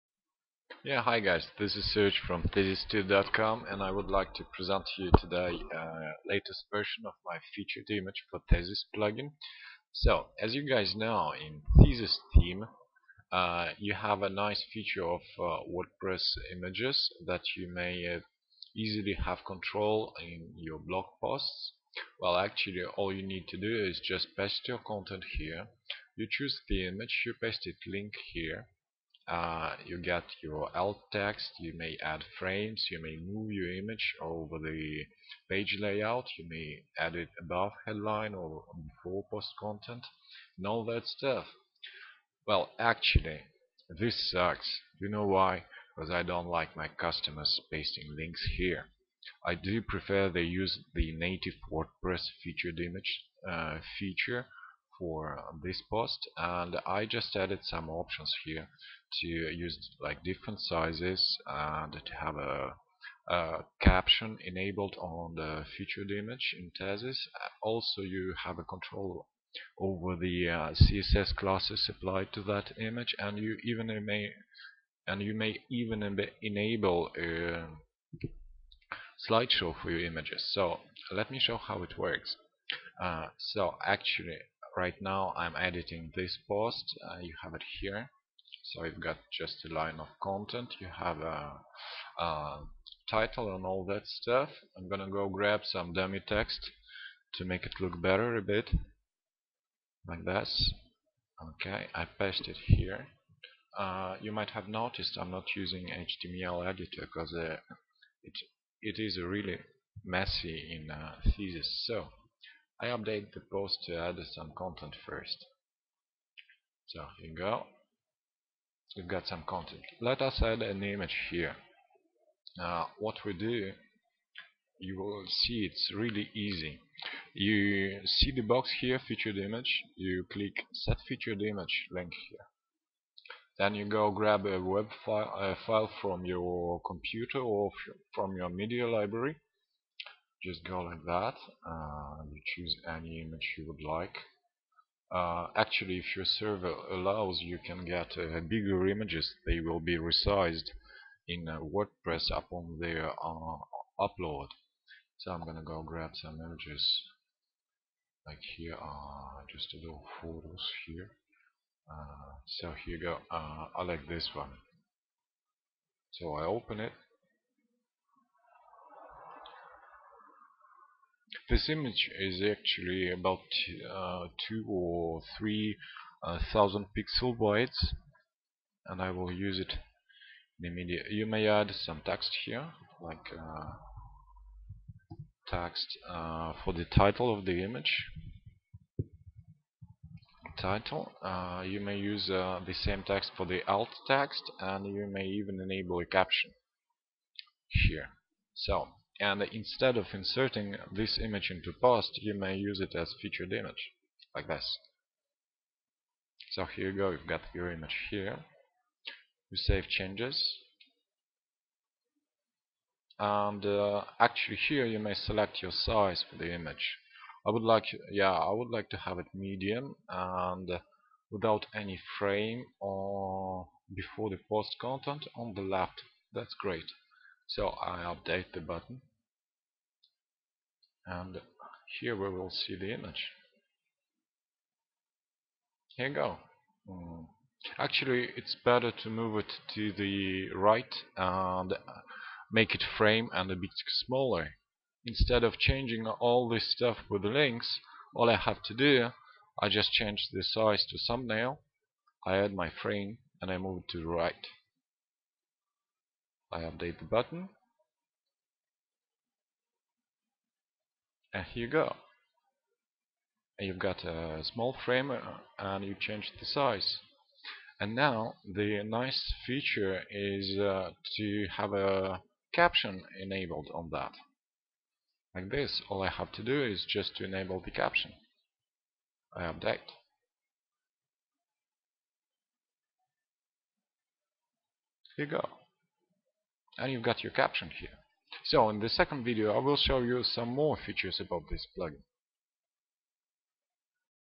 WordPress Featured Image and Thesis theme (part 1)
In this video you will see by youself that you may have thesis post image interface applied to wordpress featured image.

Wordpress featured image for thesis plugin gives you even more flexibility and without any head ache.

You will also see how easy it is to set up properly thesis post image and thumbnail only by clicking buttons.